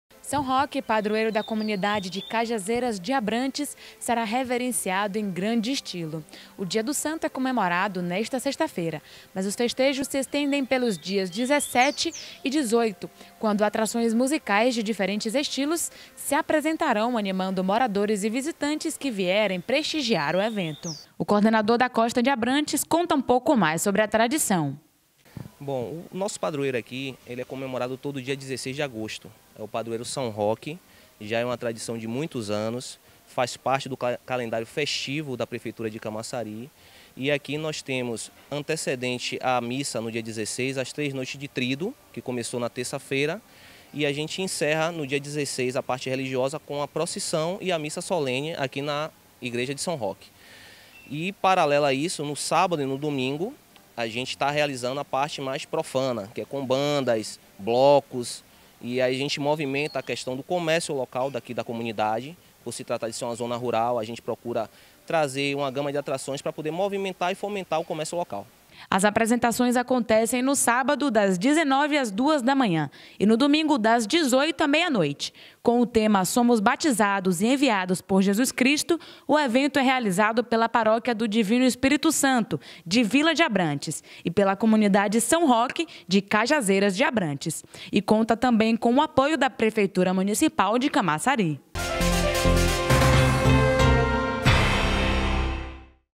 comemorações à São Roque
As comemorações à São Roque, padroeiro de Cajazeiras de Abrantes, acontece nesse fim de semana. Para saber mais detalhes da programação confira a matéria na íntegra: